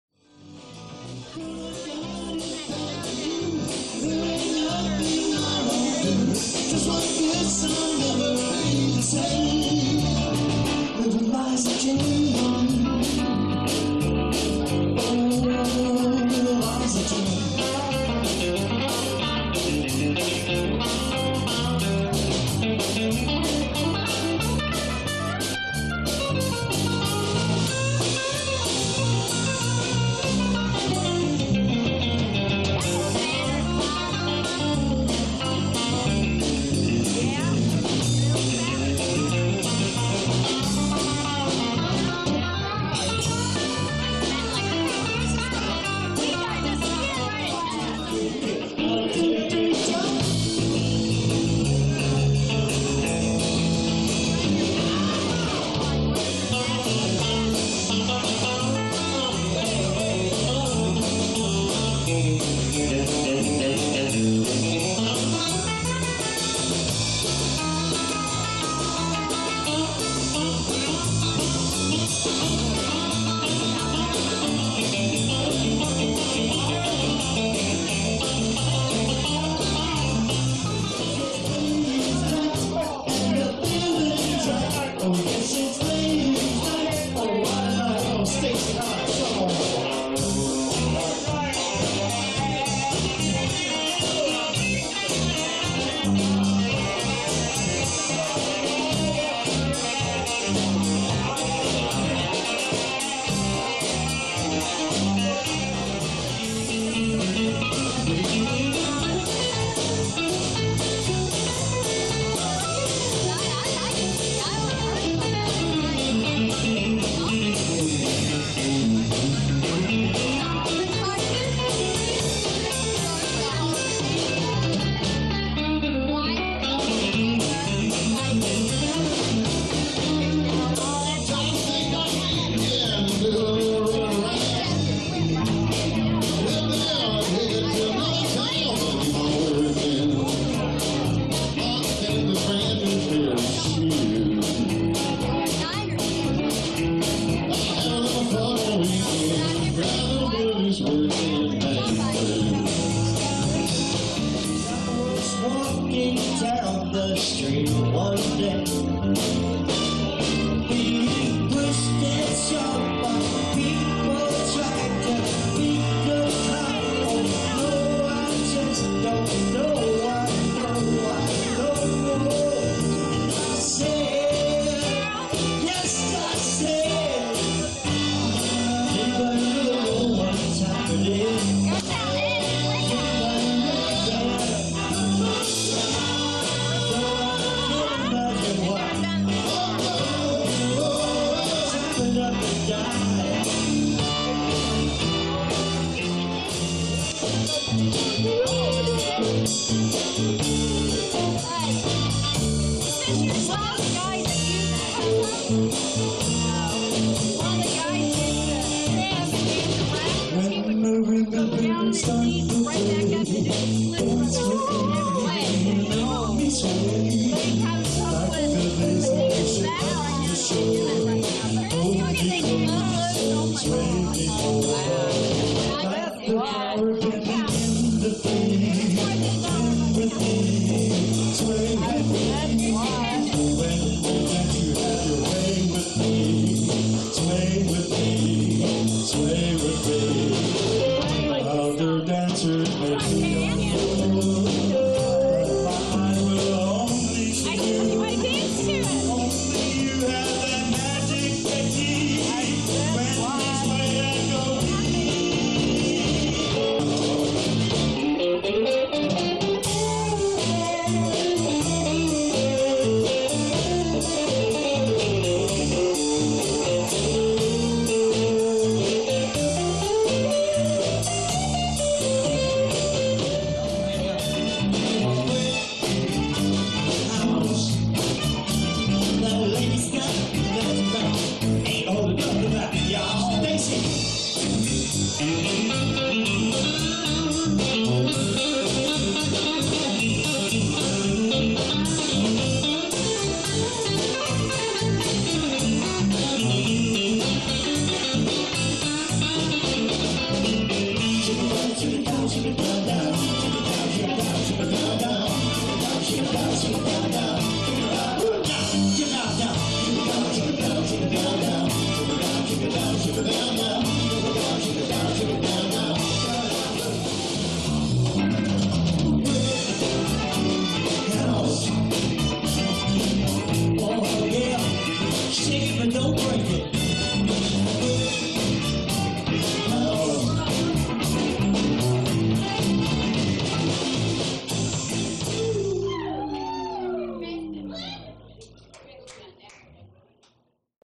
The Valley Brothers Fast Mix.avi
A short compilation of dance tunes we perform.